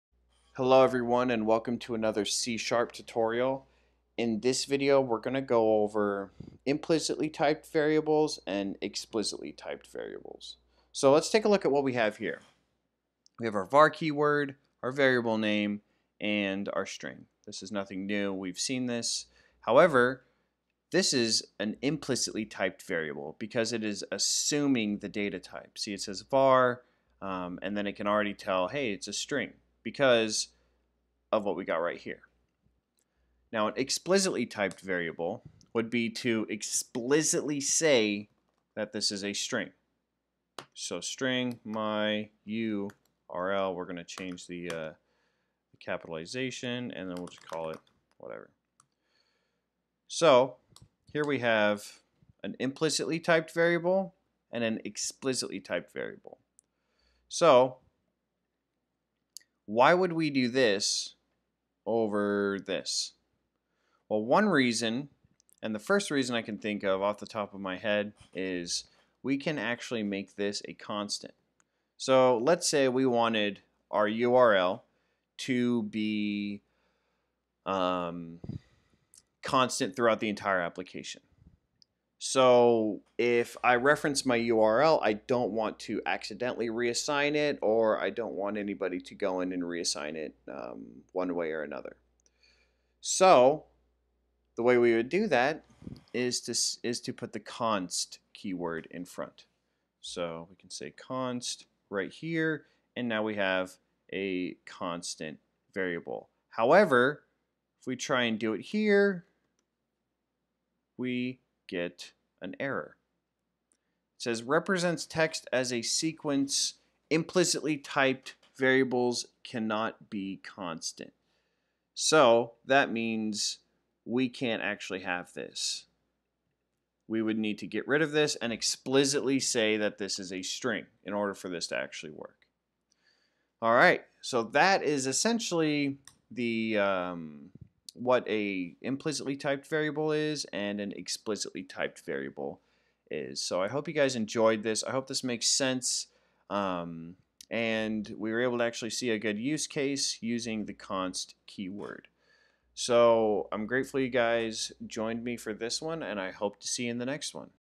C# Implicit/Explicit Variables
In this video I show you the difference between implicit and explicit variables in C# and how to implement them